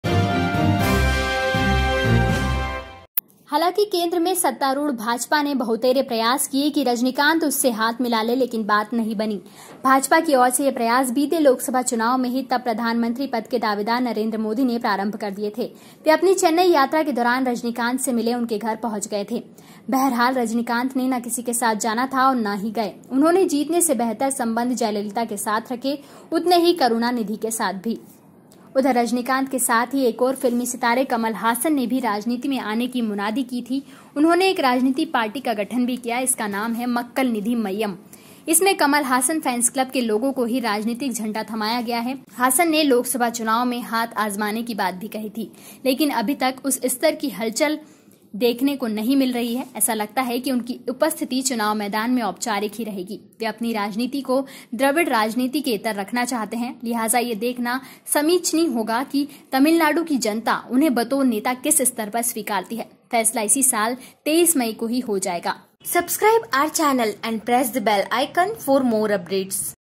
Narendra Modi घर गए फिर भी नहीं माने Rajnikant
भाजपा के साथ नहीं आएंगे रजनीकांत |

In Tamil Nadu, the color of the Lok Sabha elections has started to freeze. However this time the color of the film is not included in this color. J. The heroine of the films After Jayalalitha's death, Rajinikanth, the Bollywood superstar of Bollywood, from South India, accepted the invitation to join politics, but did not start political activities. Even refused to fight the Lok Sabha elections. They will start their innings from the assembly elections.

However, in the center, the ruling BJP made many efforts that Rajinikanth got involved with him, but the matter was not discussed. This effort by the BJP had started in the last Lok Sabha elections only after the prime ministerial candidate Narendra Modi started. He reached home to meet Rajinikanth during his visit to Chennai. Nonetheless, Rajinikanth had to go with no one and neither did he go. They keep winning better with Jayalalitha, even with Karunanidhi.